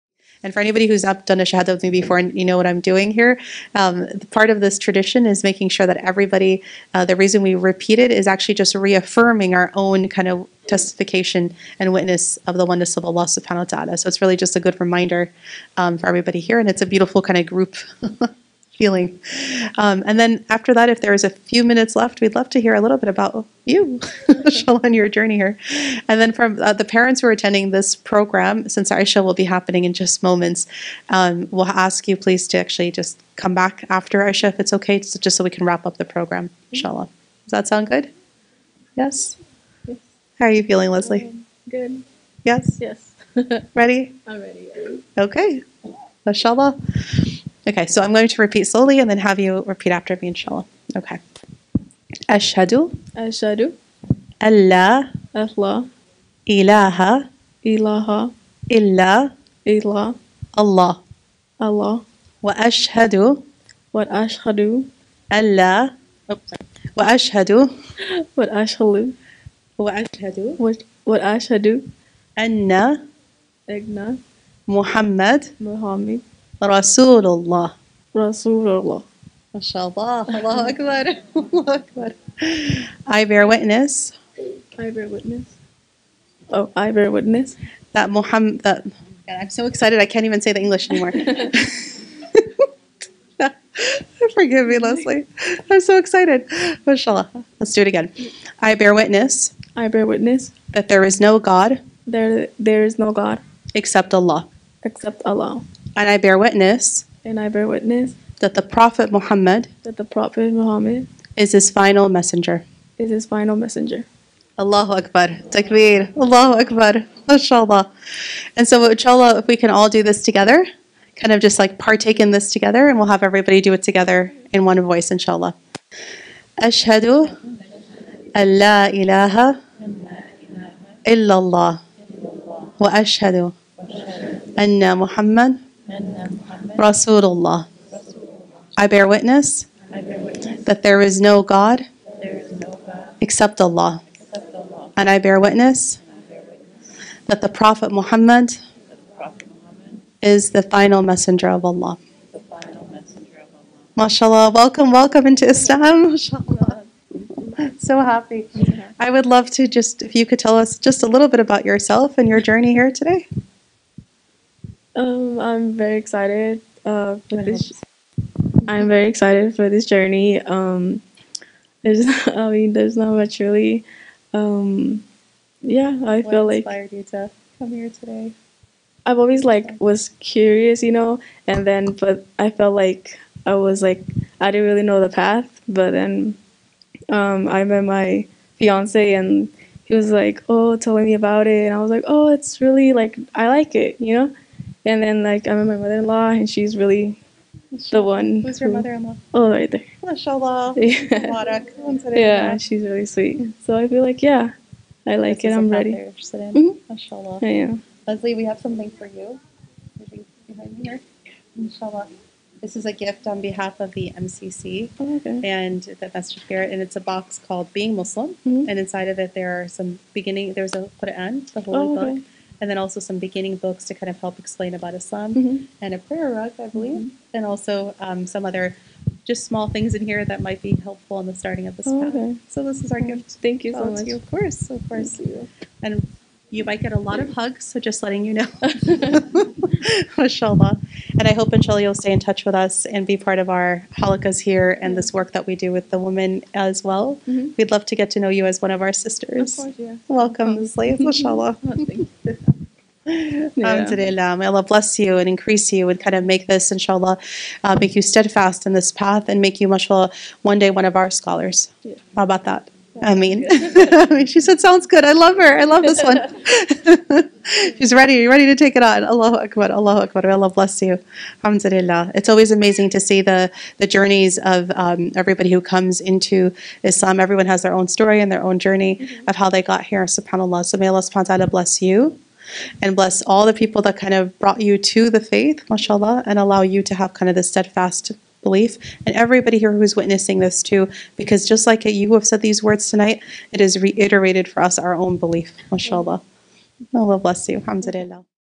Leslies' Shahadah (Declaration of Faith) | Ustadha Dr. Rania Awaad
Sr. Leslies' takes his Shahadah (Declaration of Faith) with Dr. Rania Awaad after her weekly Friday night Halaqa on Friday, October 6, 2023.

If you are a new Muslim in the San Francisco Bay Area who would like to receive updates from MCC for new Muslims, please